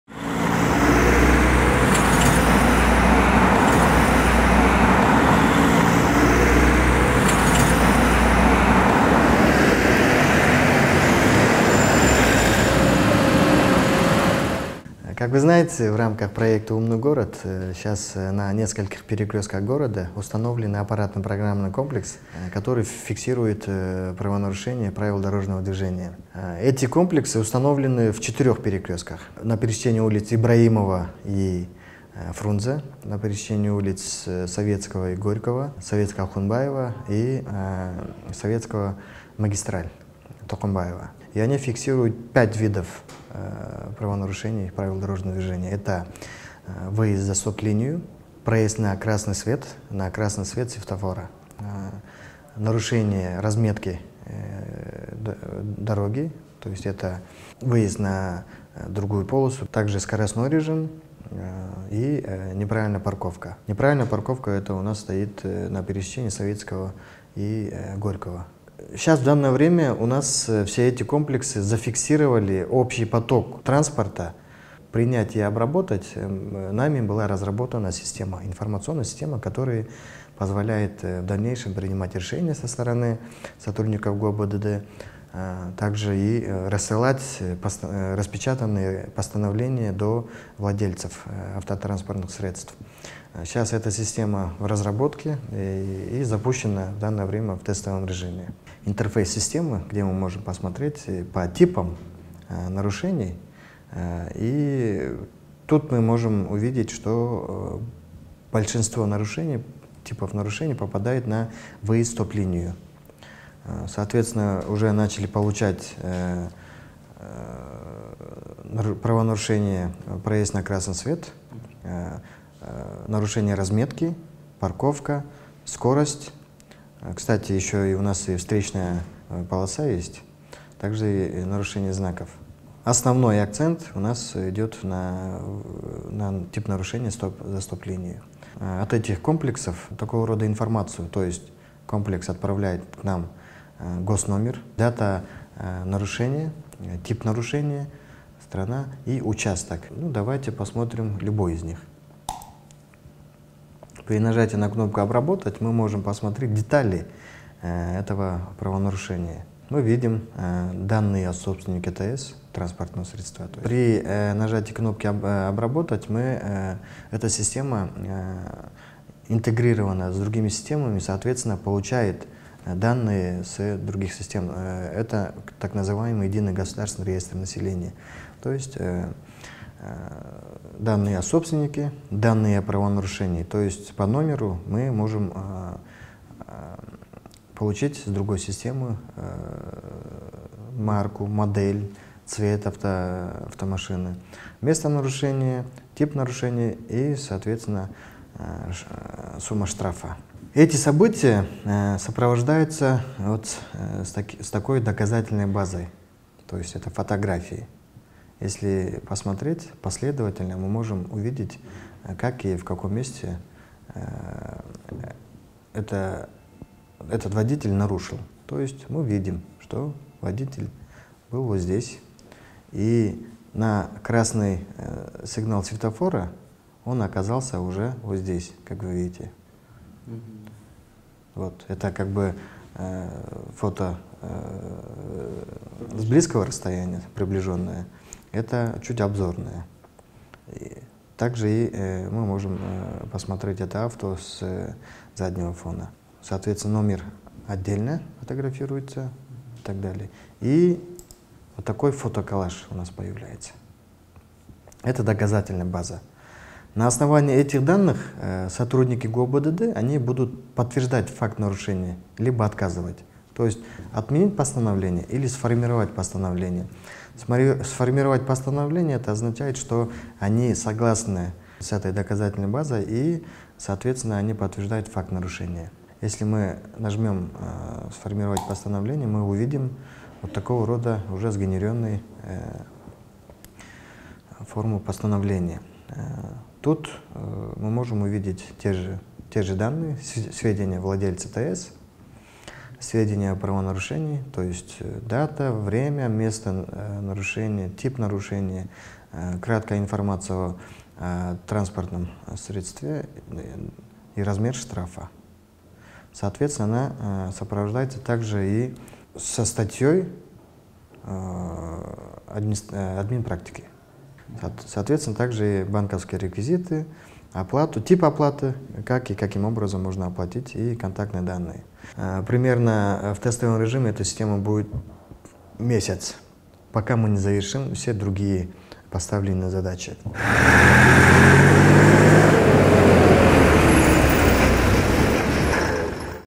"Умный город" и штрафы. Как это работает?
В рамках проекта «Умный город» на нескольких перекрестках города установлены программные комплексы, которые фиксируют нарушения правил дорожного движения.
Они установлены на пересечениях улиц Ибраимова-Фрунзе, Байтик Баатыра-Горького, Байтик Баатыра-Ахунбаева и Байтик Баатыра-Масалиева.
Они запущены в тестовом режиме. Они фиксируют 5 видов нарушений: «Выезд за стоп-линию», «Проезд на красный свет светофора», «Нарушения разметки», «Скоростной режим» и «Неправильная парковка